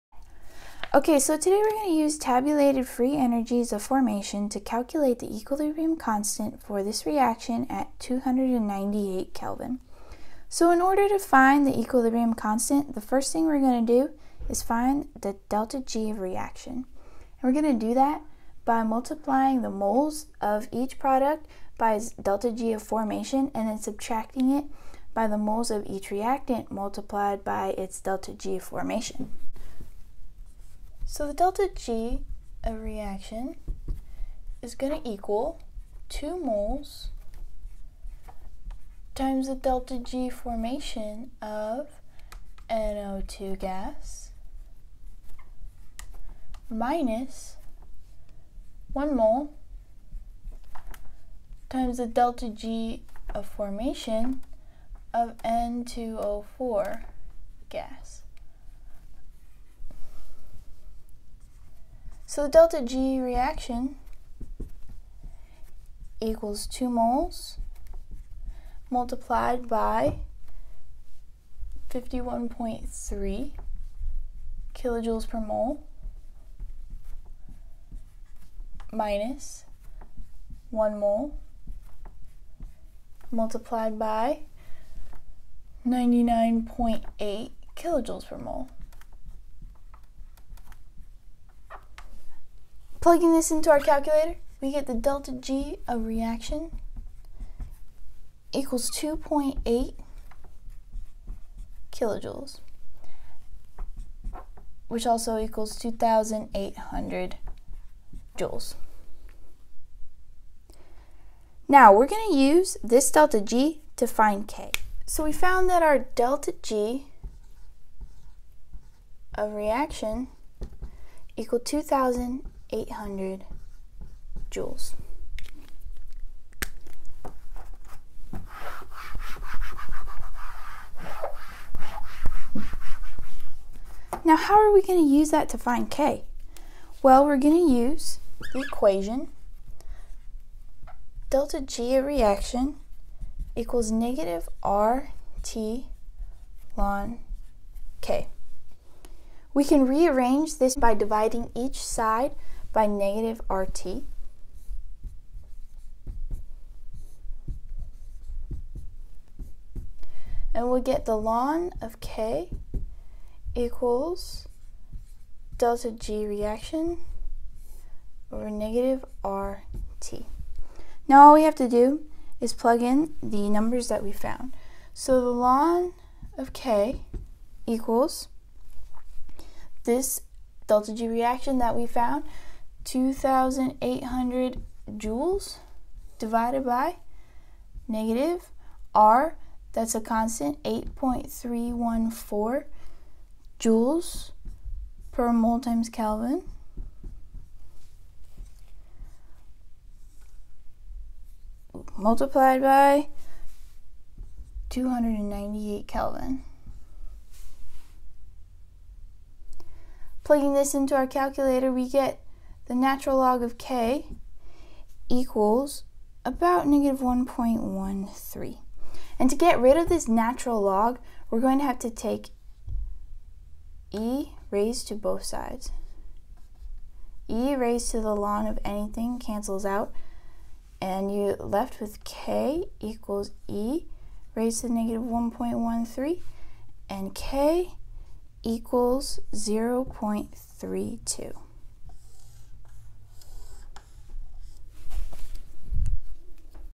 How to Calculate the Equilibrium Constant, K, By Finding Delta G
This video is about how to use tabulated free energies of formation to calculate the equilibrium constant for a reaction at 298 K.